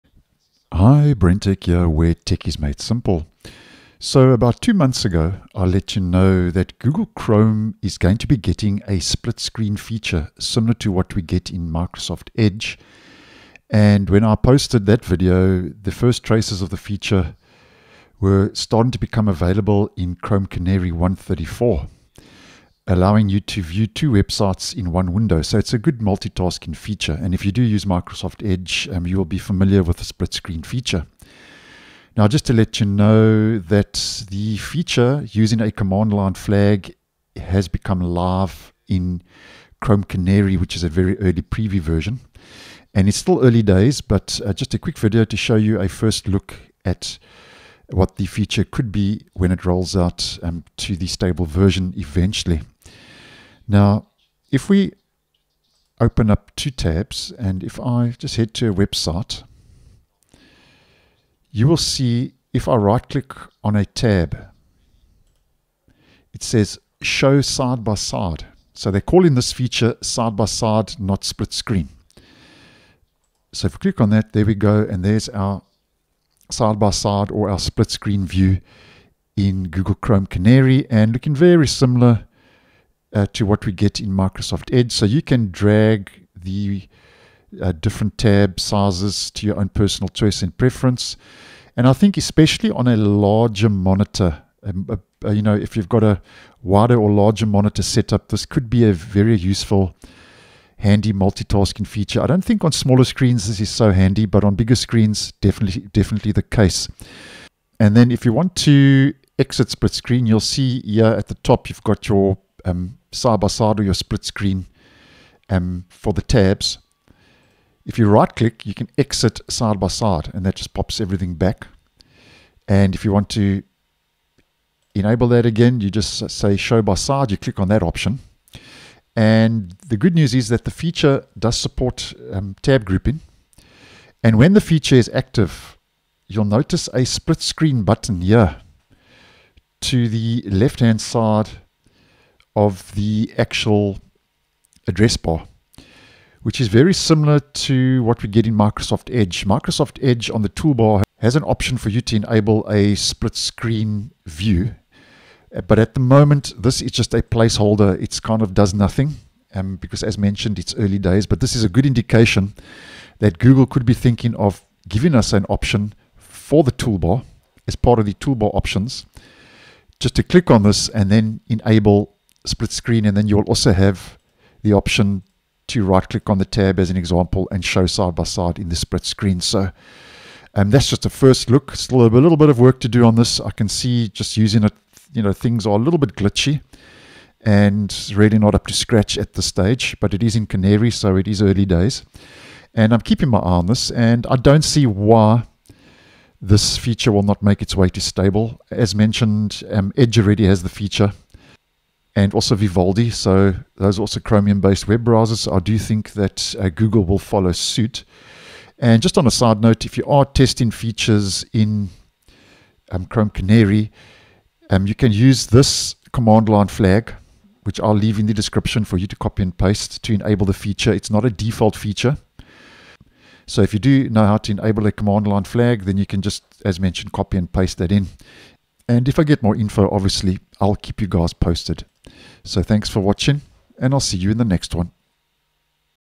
First Look at Google Chrome's New Split Screen Feature
Google is working on a Split Tabs feature, similar to the Split Screen functionality already available in Microsoft Edge and Vivaldi

--enable-features=SideBySide